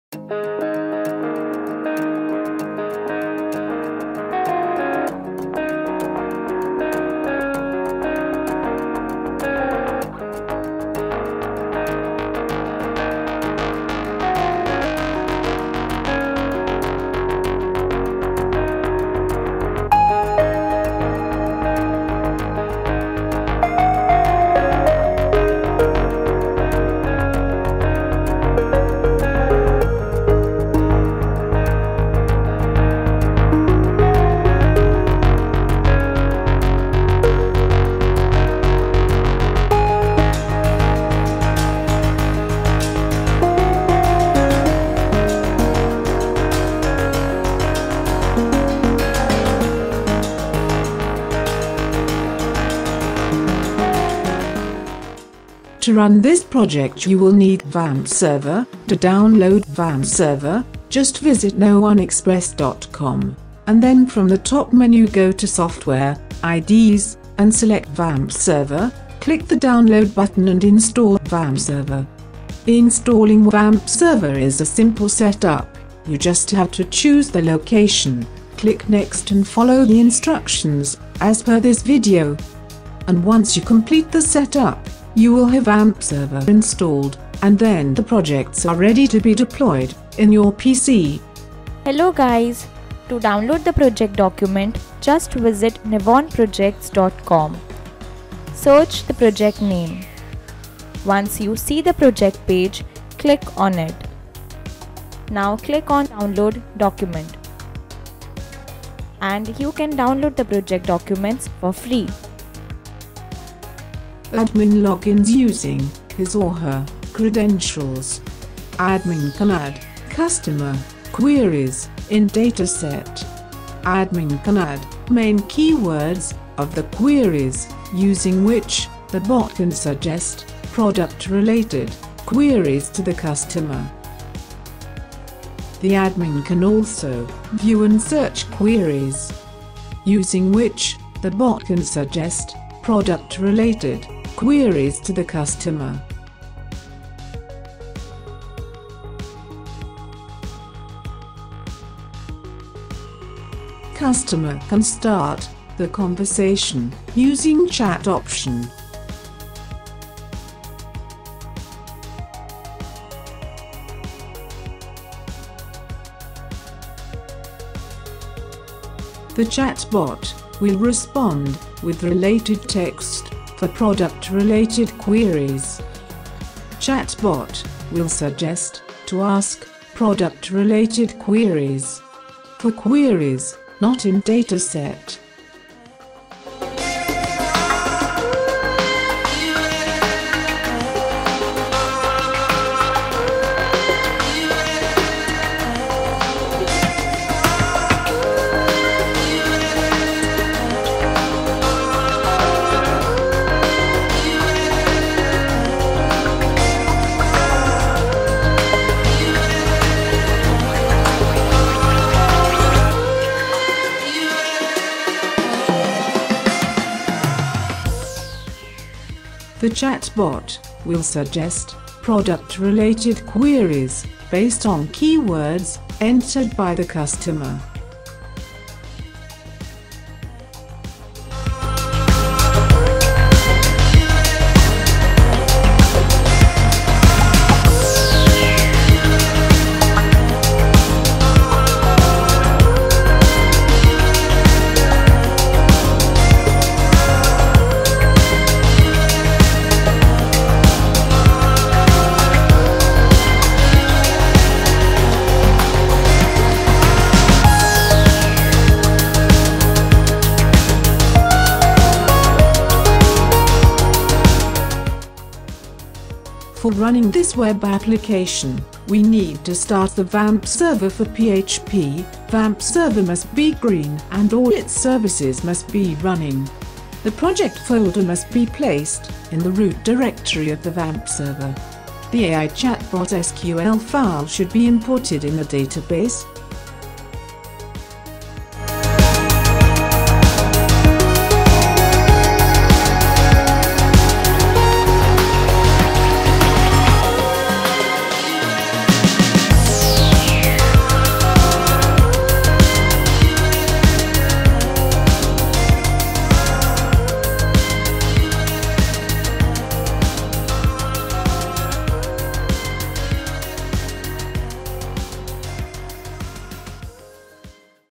Making Ecommerce Chatbot For Handling Ecommerce Customer Support
This video shows the demonstration of “ Online Logistic Chatbot System”. This Logistic Chatbot System will provide 24/7 Customer support for all Ecommerce transactions.

Chatbot are usually stateful services remembering previous commands. A chatbot is a type of software that conducts a conversation via auditory or textual methods. Most of us have interacted with chatbots while online shopping or planning a holiday. Logistic includes tangible goods such as material equipment and supplies. Logistics can be benefited using chatbots by improving customer-facing operations and supply chain operations. Chatbots do not make mistakes and they are intuitive and easy to use. There are two modules in this online logistics system namely, Admin and Chatbot. Chat where we can chat using AI chatbot. Admin can login using credentials. There you can add questions and give three main keywords. We can enter the message by typing “hi” where AI chatbot will reply with hello greetings. As per the question asked, it will respond accordingly.